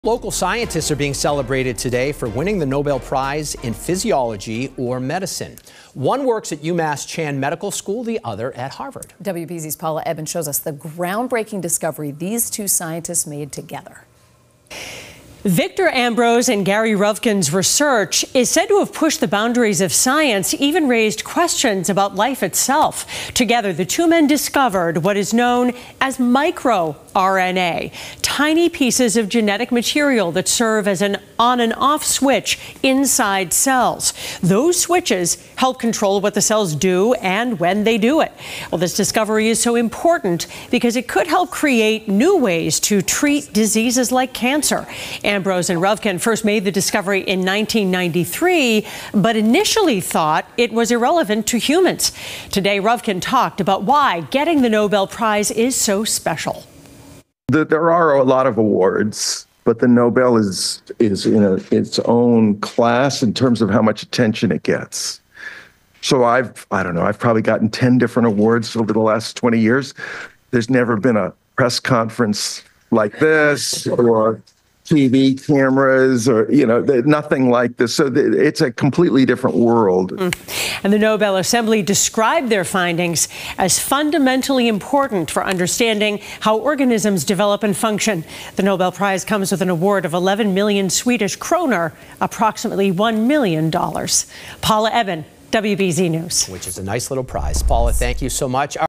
Two Massachusetts residents receive Nobel Prize in medicine for microRNA discovery
Two researchers working in Massachusetts have been awarded the Nobel Prize in medicine for their discovery of microRNA.